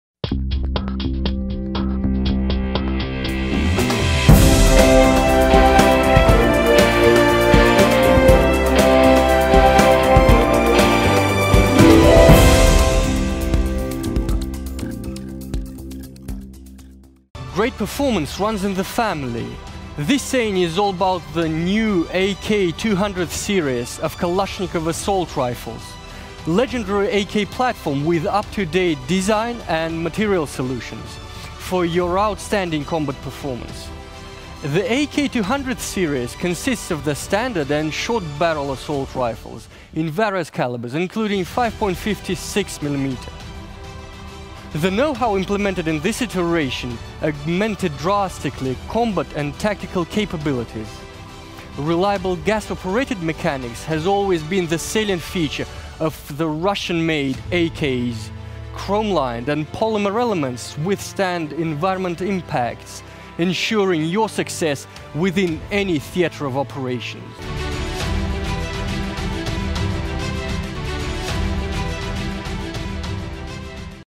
AK-200
Functional and accurate Kalashnikov assault rifles of the 200th series will be appreciated by fighters of any divisions for their high tactical and combat characteristics. Versions with a shortened barrel and a folding stock (AK-204, AK-205) represent advanced Russian solutions in the field of small arms in a compact design for convenience and mobility on the battlefield.